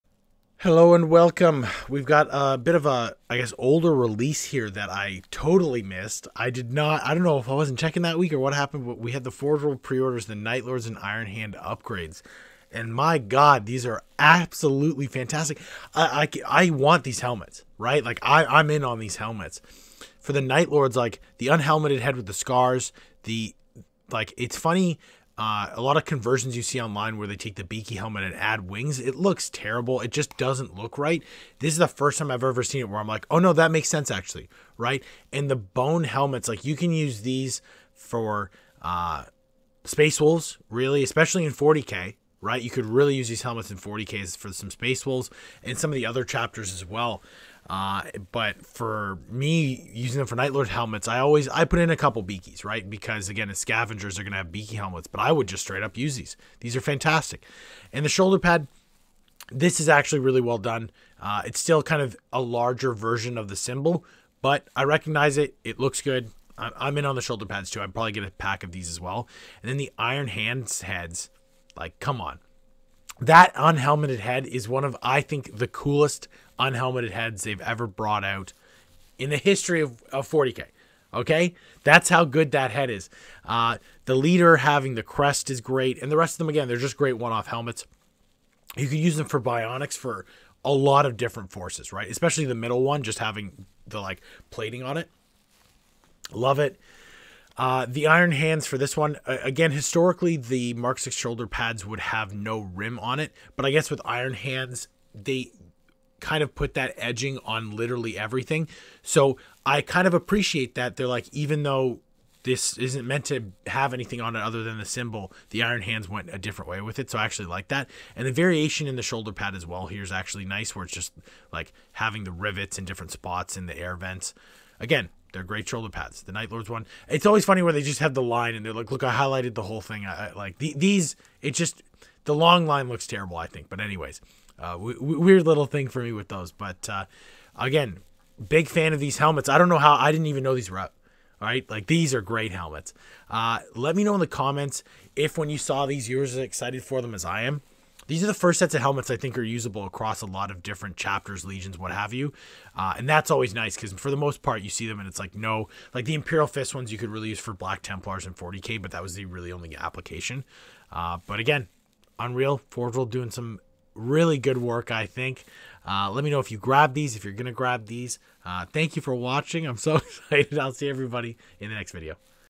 Forge World Night Lords and Iron Hands Conversion Bits
I totally missed this release, and I have to say these are definitely my favorite heads so far.